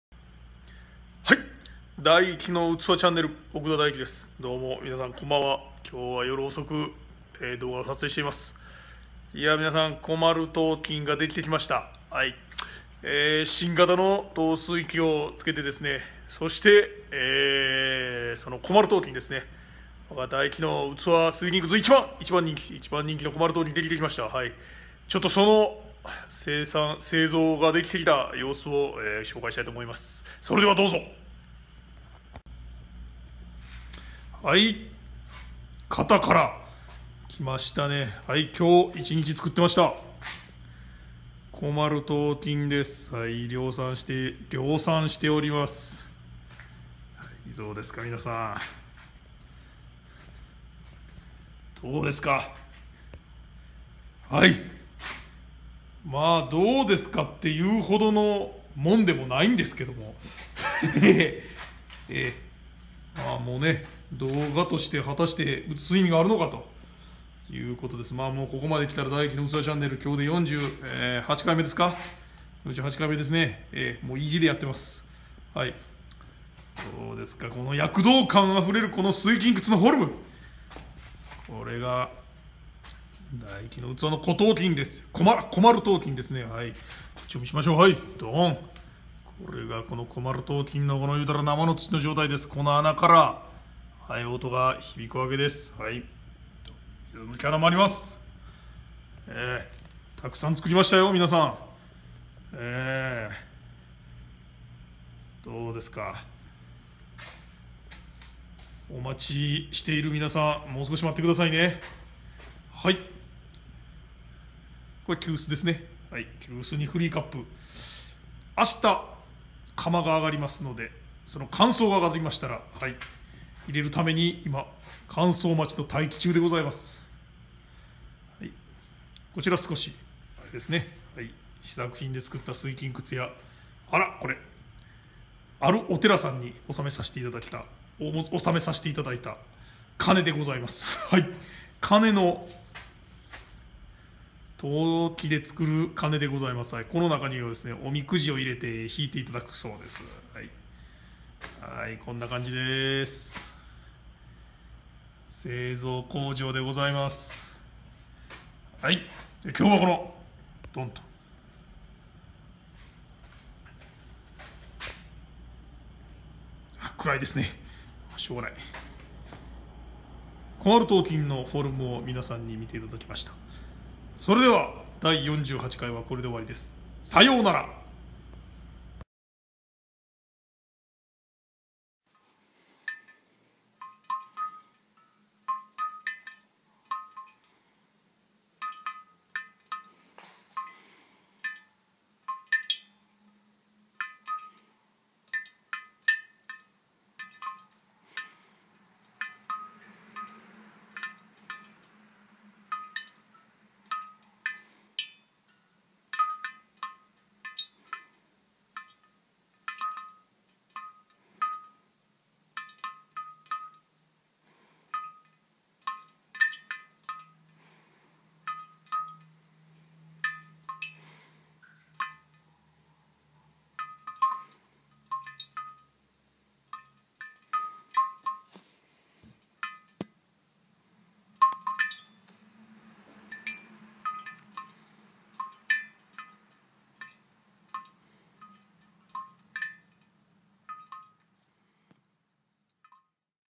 【大器の器Ch】#48_170512 「小丸陶琴製造現場」
これが製造現場です。
水琴窟です。
乾燥させます。

詳しいブログの内容はこちら。